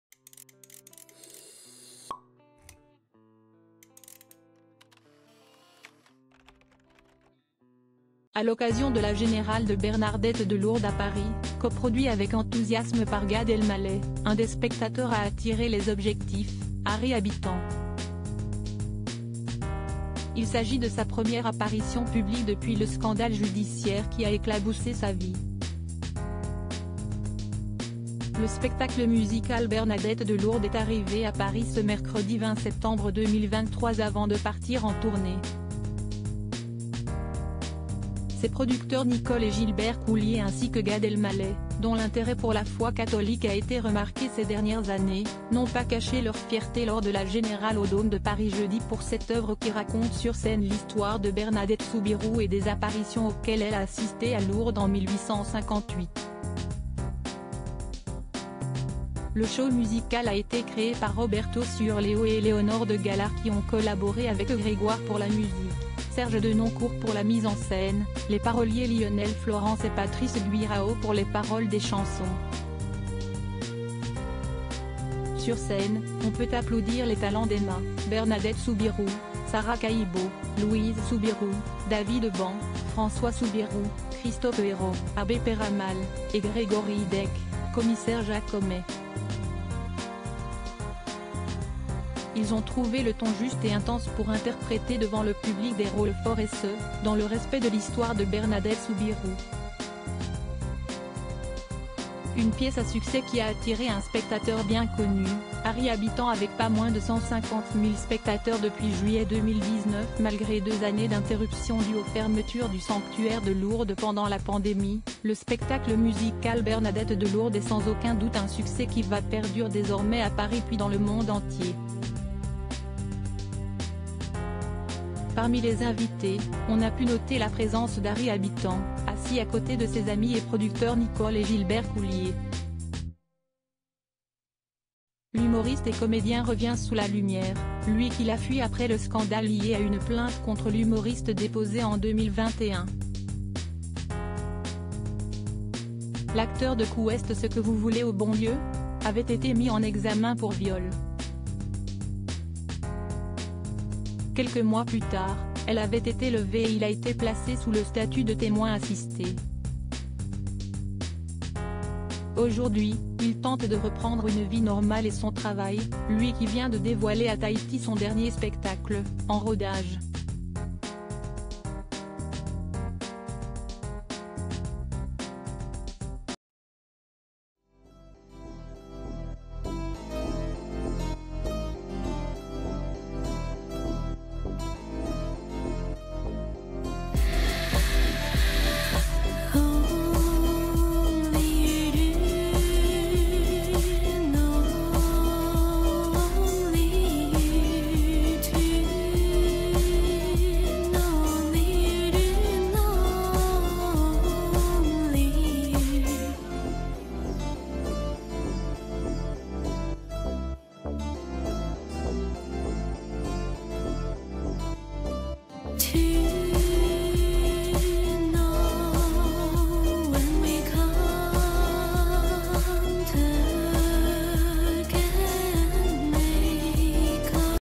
Gad Elmaleh fier de présenter à Paris Bernadette de Lourdes, première apparition publique d'Ary Abit
Gad Elmaleh fier de présenter à Paris Bernadette de Lourdes, première apparition publique d'Ary Abittan